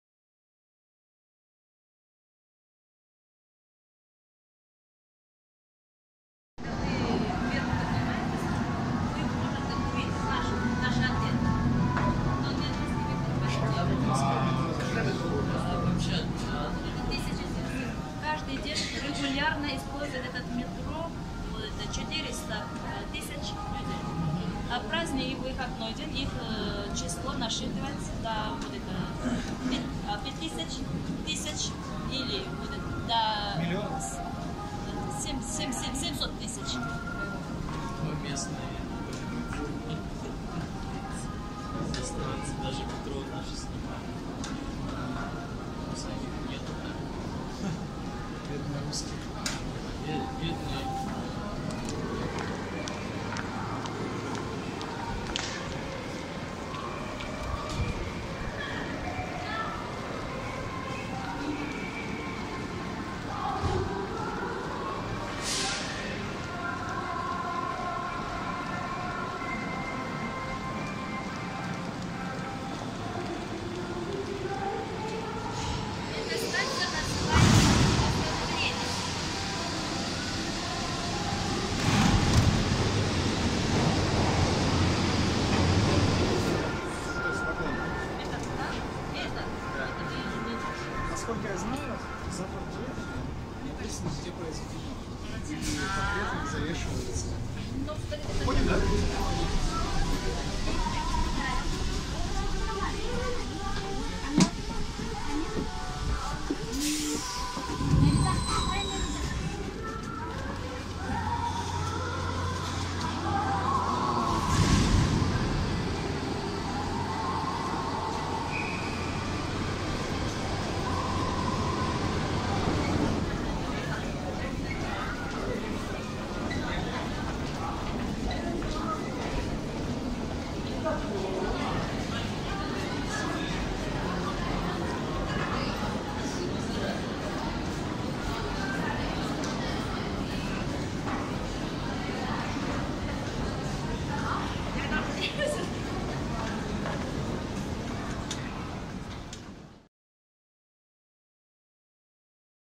Метро Пхеньяна
Говорят, в Северной Корее многое запрещают фотографировать. Побывала в метро Пхеньяна, успела кое-что снять смартфоном.